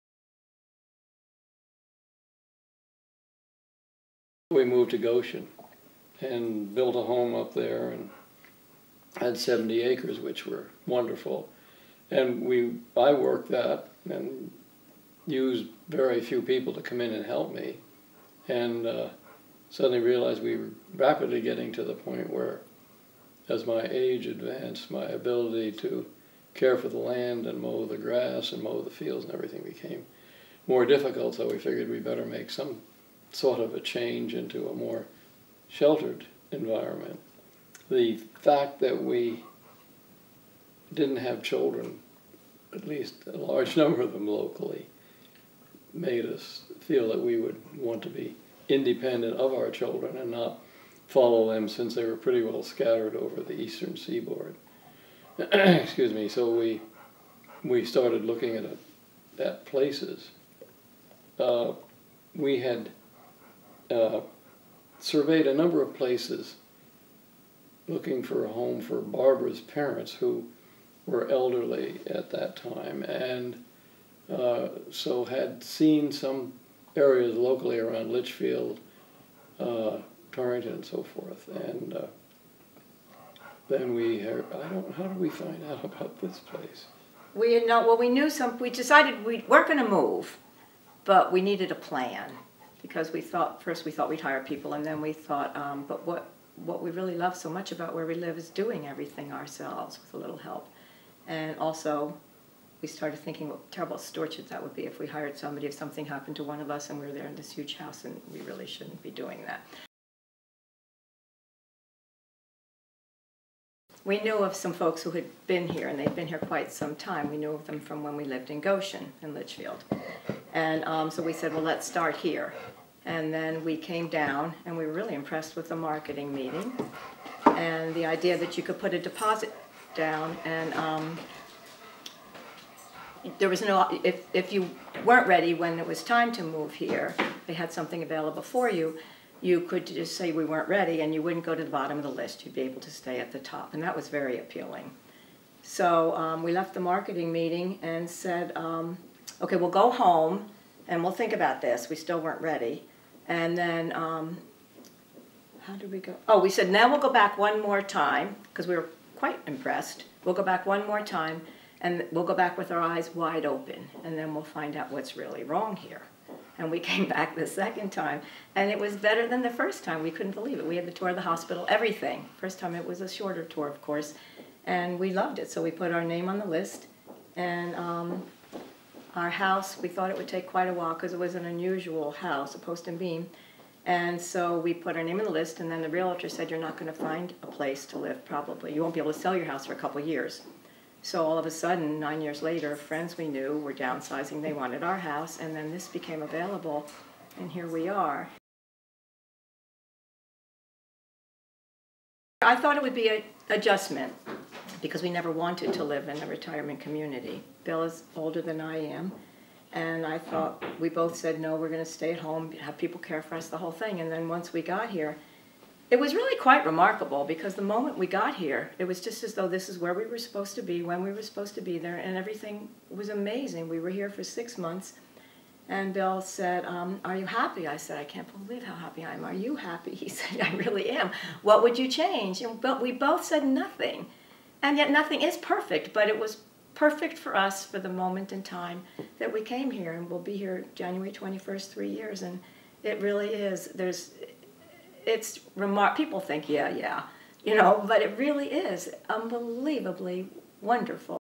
Masonicare Resident Testimonial of The Hillis'- "Process of moving to Ashlar Village"
Dr. William and Barbara Hillis talk about their journey to moving to Masonicare at Ashlar Village.